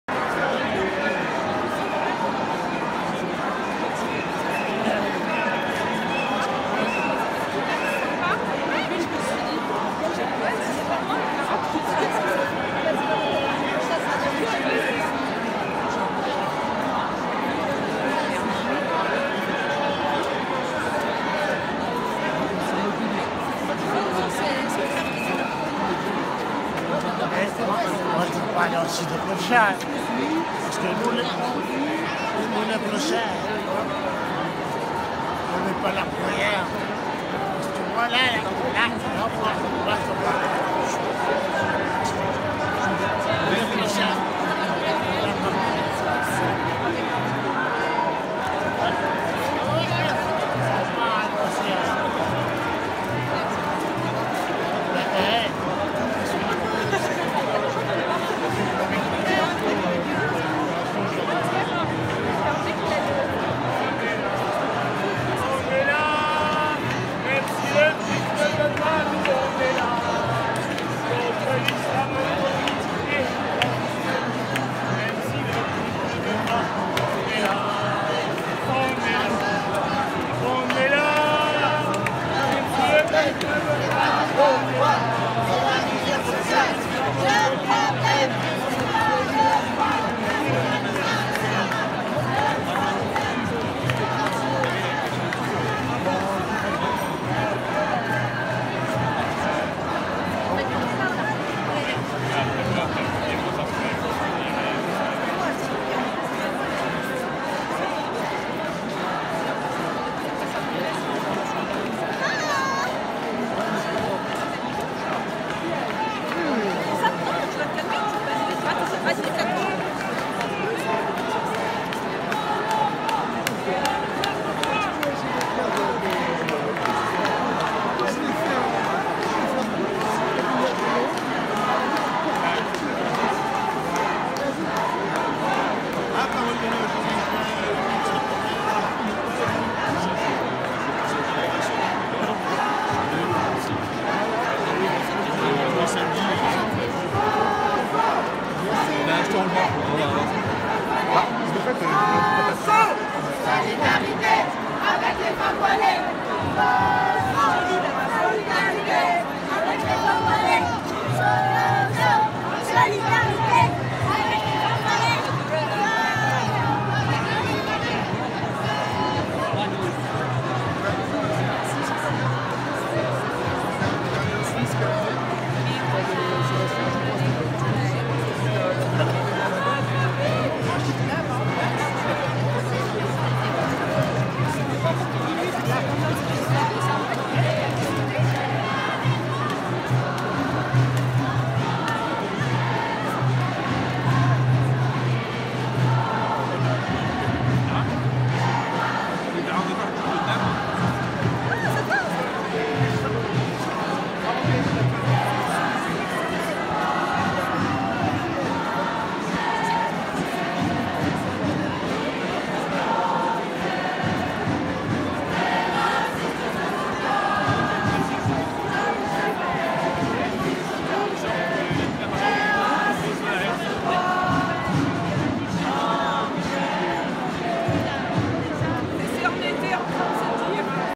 Manifestation contre l'islamophobie
Paris le 10 novembre 2019, plus de 13 000 personnes marchent à Paris contre l’islamophobie